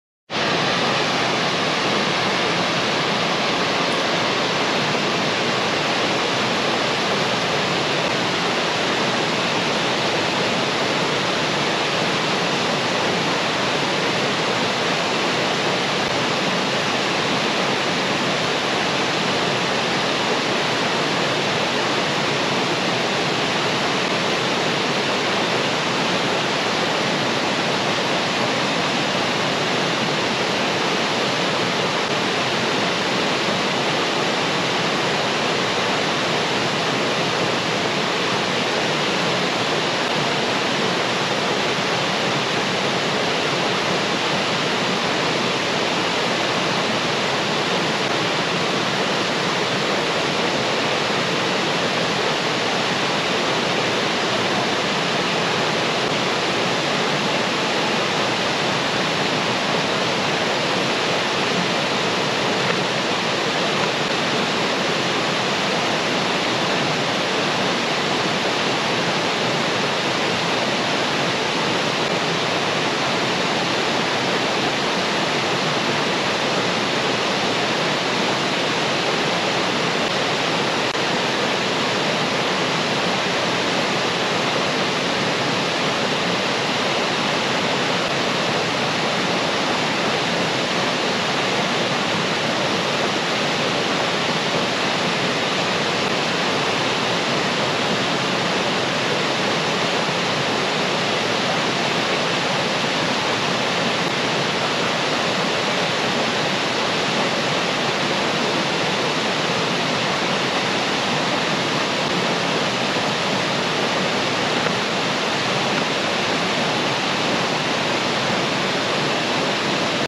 Enders
2 minute capture of the Enders middle waterfall.